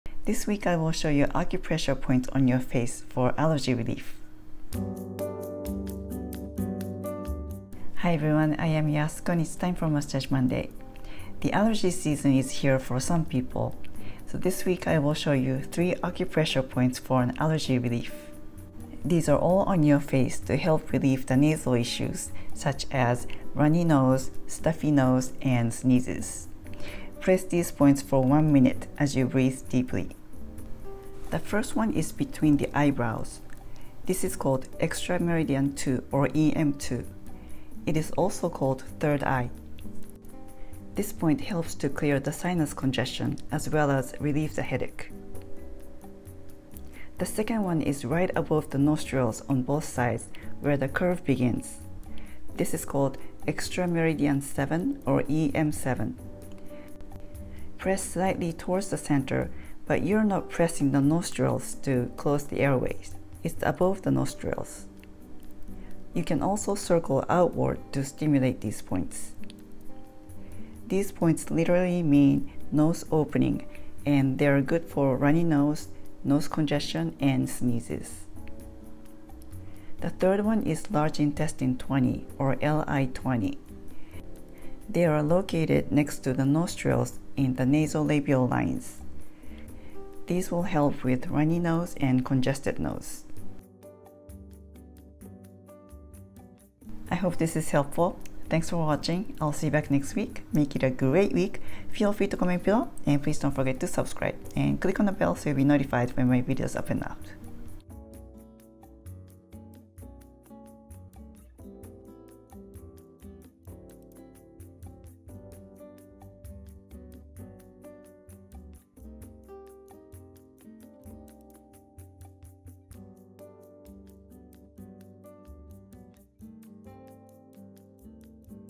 Acupressure Points for Allergy Relief
This week I will show you some acupressure points on your face for allergy relief.  (Massage Monday 544)

Amazon Store